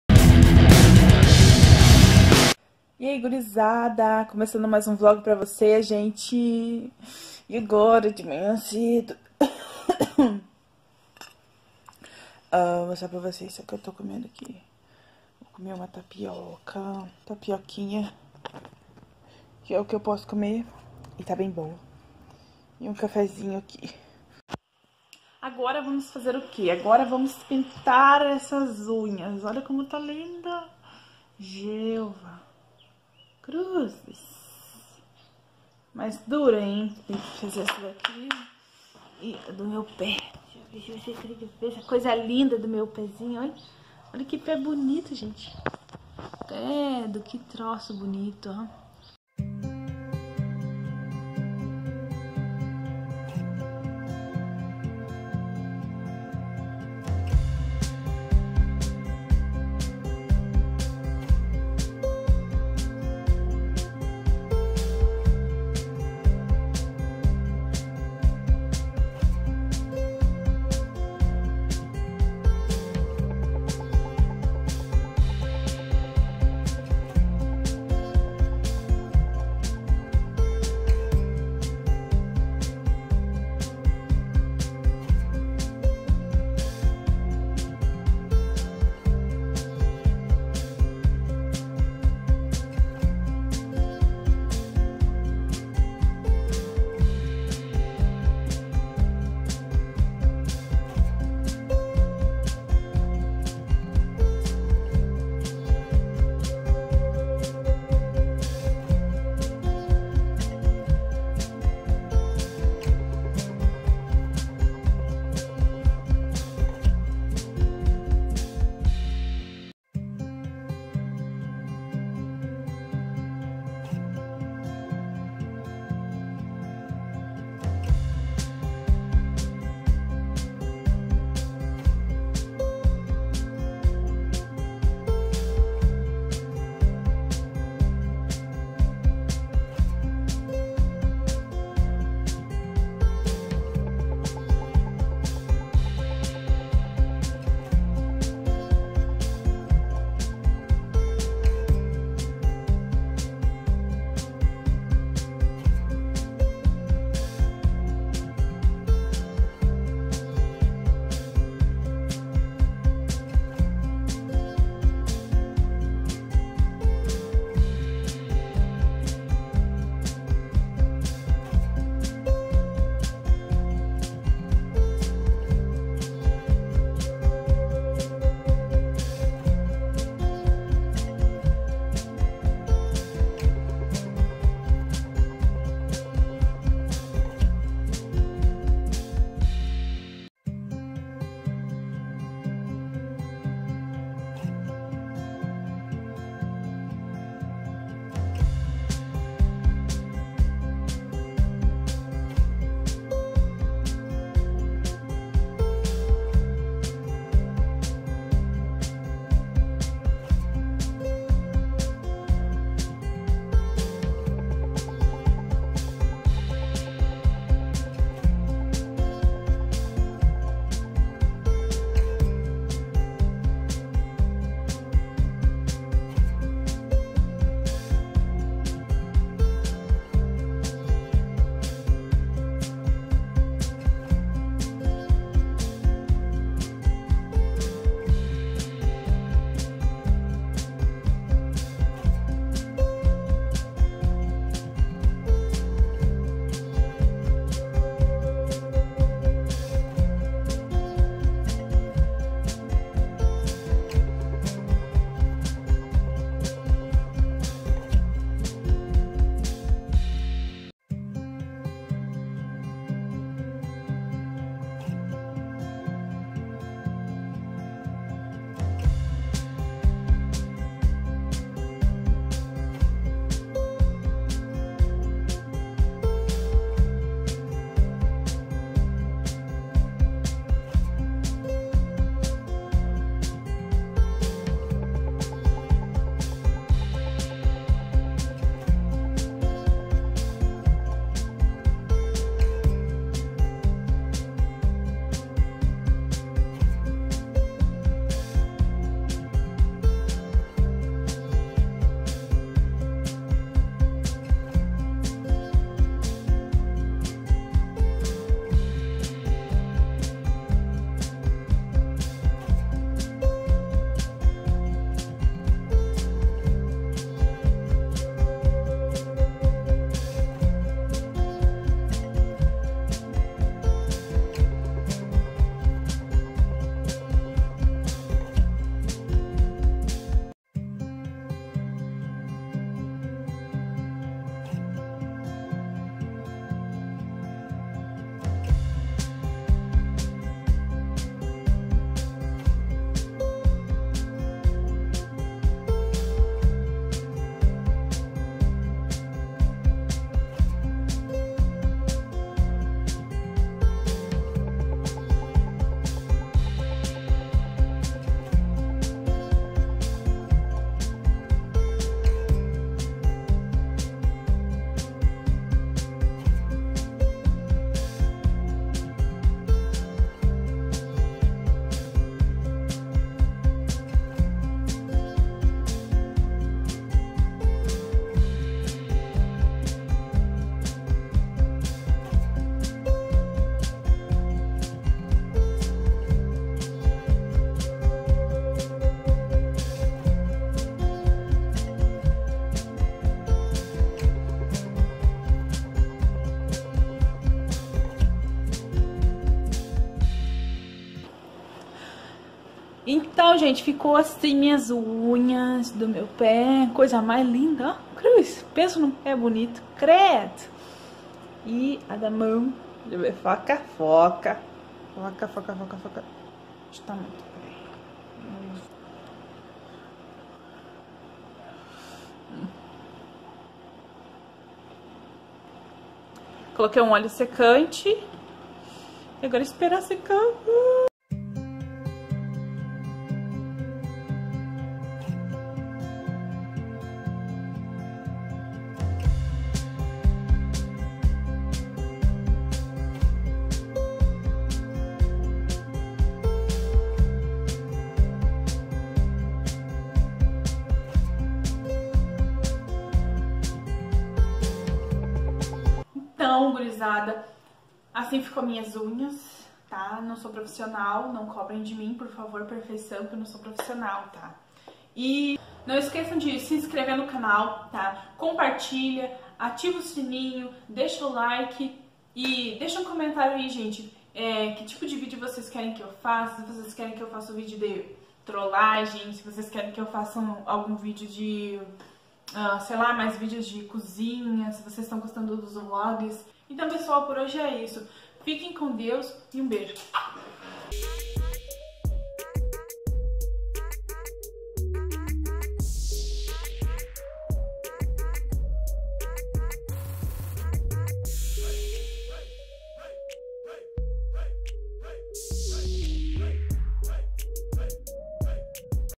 PINTANDO AS UNHAS!
♦ Oi gurizada!!!
Pintando as unhas do pé e da amo,

VÍDEO NOVO TODAS AS SEGUNDAS, QUARTAS E SEXTAS!